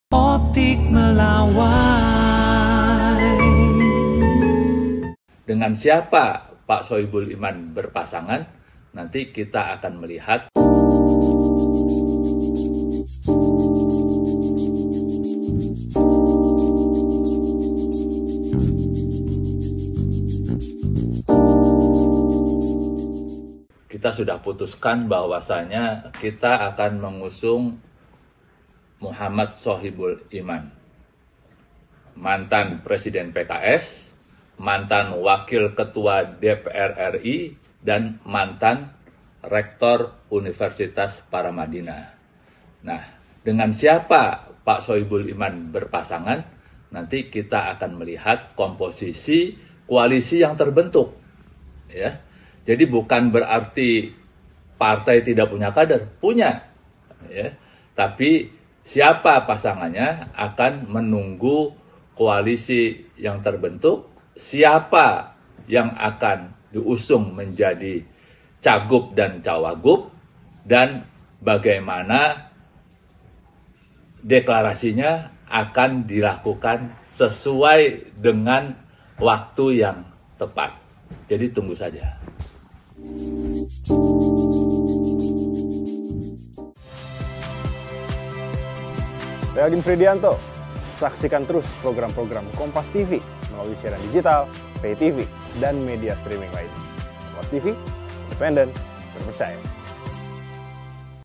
Soal Pendamping Sohibul Iman di Pilkada Jakarta, Begini Respons PKS
JAKARTA, KOMPAS.TV - Partai Keadilan Sejahtera (PKS) telah mengajukan nama kadernya, Mohamad Sohibul Iman, sebagai bakal calon gubernur untuk Pilgub Jakarta 2024, sementara pendampingnya akan ditentukan setelah koalisi terbentuk.

Juru Bicara PKS Ahmad Mabruri menjelaskan keputusan bakal calon wakil gubernur untuk menemani Sohibul, akan diumumkan saat deklarasi resmi partai yang disebut bakal dilakukan dengan partai politik lain.

"Pada rapat kemarin kita sudah putuskan bahwasannya kita akan mengusung Muhammad Sohibul Iman, mantan Presiden PKS, mantan Wakil Ketua DPR RI, dan mantan Rektor Universitas Paramadina," ucap Ahmad Mabruri.

Video editor: Dawud Majid